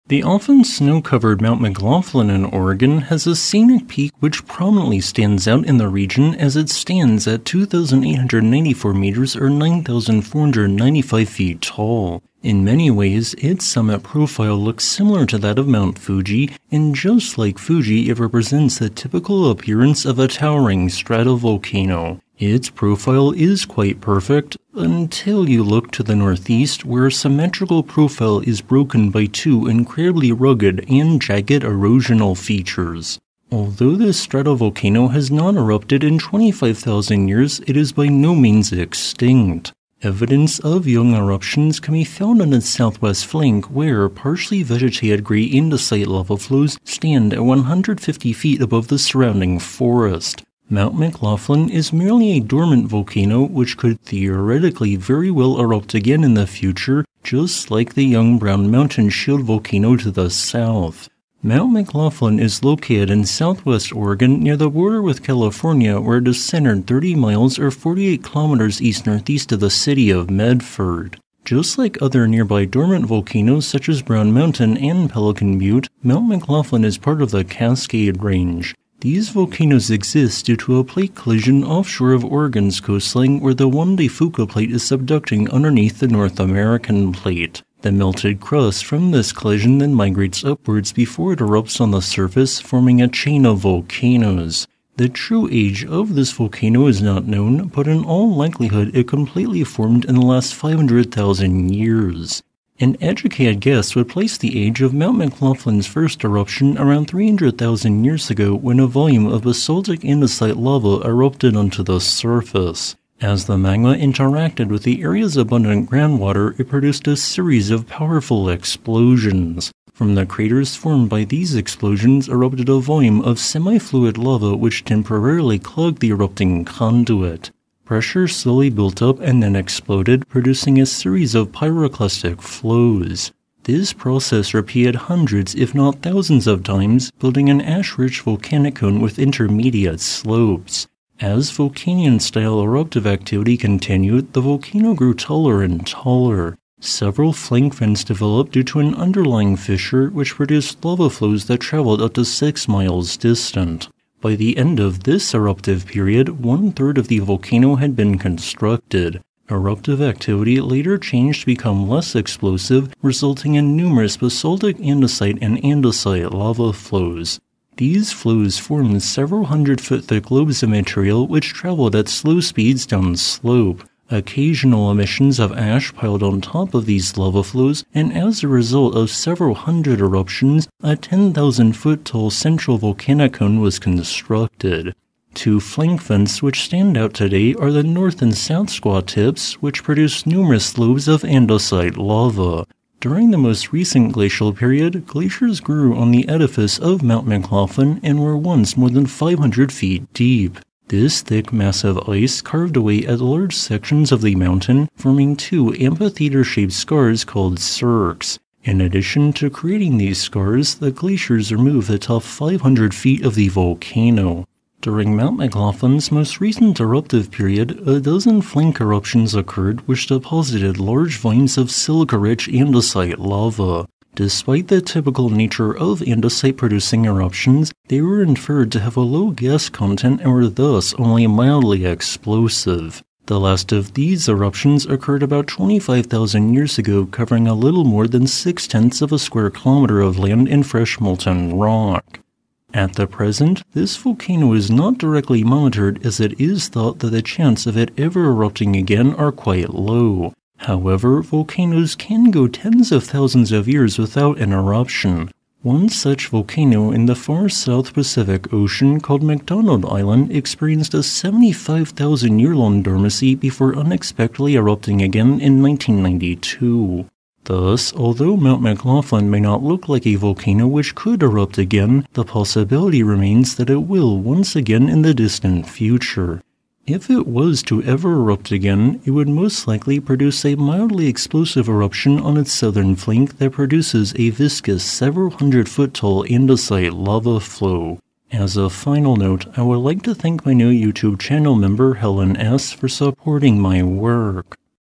The Tall Volcano in Oregon; Mount McLoughlin
The tall Mount McLoughlin is one of Oregon's more than a dozen volcanoes. Although it has been thoroughly carved by glaciers, this volcano is not extinct but merely dormant, and could throetically erupt again in the distant future. Its last eruption occurred approximately 25,000 years ago, which produced blocky andesite lava flows that flowed towards the southwest of its summit. This video will discuss Mount McLoughlin which in some ways looks similar to Mount Fuji.

Thumbnail Photo Credit: This work "MtMcLoughlin2", is a derivative of a photo (resized, cropped, text overlay, overlaid with GeologyHub made graphics (the image border & the GeologyHub logo)) from "Mt.

0:00 Mount McLoughlin
1:19 Geologic Setting
1:35 1st Eruptive Period
2:39 2nd Eruptive Period
3:05 Glaciers Carve the Volcano
3:35 3rd Eruptive Period
4:03 Long Dormant Volcanoes